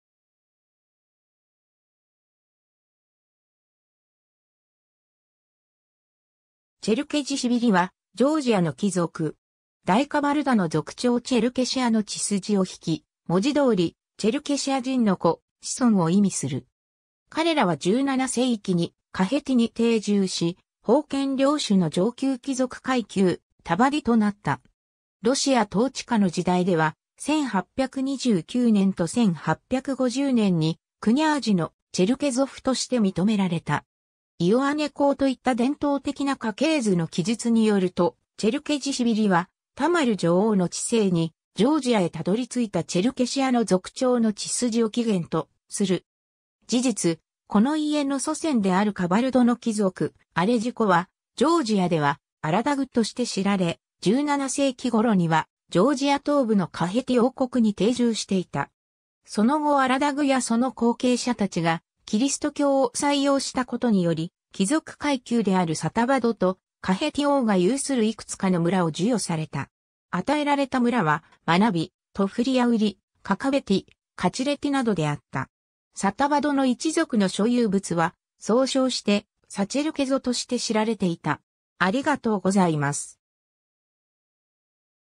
チェルケジシヴィリ
チェルケジシヴィリ（グルジア語: ჩერქეზიშვილი、グルジア語ラテン翻字: Cherkezishvili）は、ジョージアの貴族。
大カバルダの族長チェルケシアの血筋を引き、文字通り「チェルケシア人の子/子孫」を意味する。
彼らは17世紀にカヘティ（ジョージア東部）に定住し、封建領主の上級貴族階級タヴァディ（公）となった。
ロシア統治下の時代では、1829年と1850年にクニャージの「チェルケゾフ」（ロシア語: Черкезовы）として認められた。
 イオアネ公といった伝統的な家系図の記述によると、チェルケジシヴィリはタマル女王の治世 (1184–1213) にジョージアへ辿り着いたチェルケシアの族長の血筋を起源とする。
事実、この家の祖先であるカバルドの貴族アレジュコは、ジョージアではアラダグとして知られ、17世紀頃にはジョージア東部のカヘティ王国に定住していた。
その後アラダグやその後継者たちがキリスト教を採用したことにより、貴族階級であるサタヴァドと、カヘティ王が有するいくつかの村を授与された。
与えられた村は、マナヴィ、トフリアウリ、カカベティ、カチレティなどであった。
サタヴァドの一族の所有物は、総称してサチェルケゾ (საჩერქეზო) として知られていた。